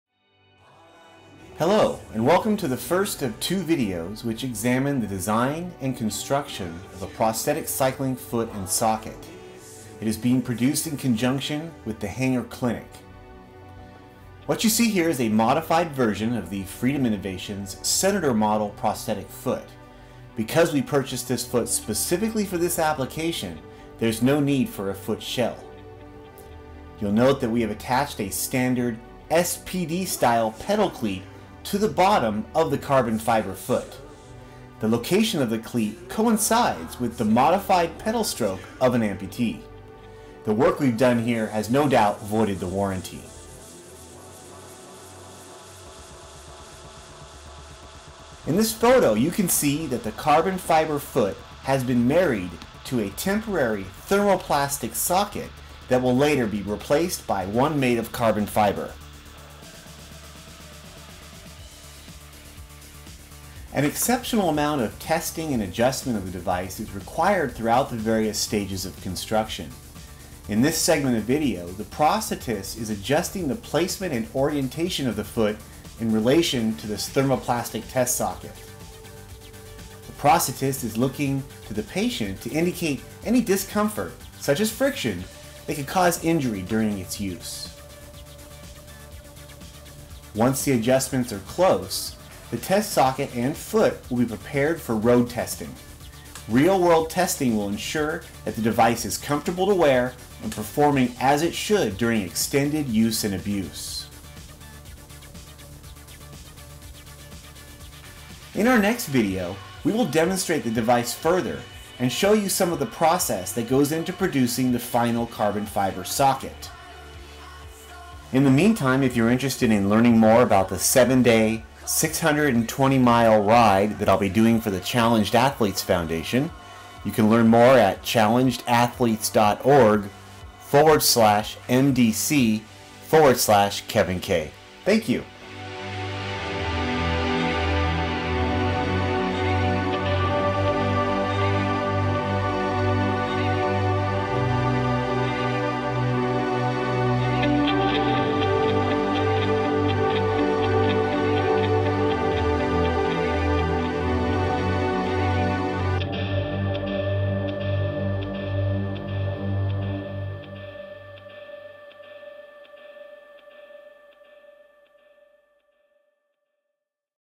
Prosthetic Cycling Foot Design Video Part 1
Kevin Korenthal demonstrates the modification of an off-the-shelf prosthetic foot for use as a full-time cycling foot. Part 1 of 2. The device is being built to accommodate Mr. Korenthal's aspirations to complete the Dodge Million Dollar Challenge, a 7-day, 620 mile coastal bike ride from San Francisco to San Diego. for more information.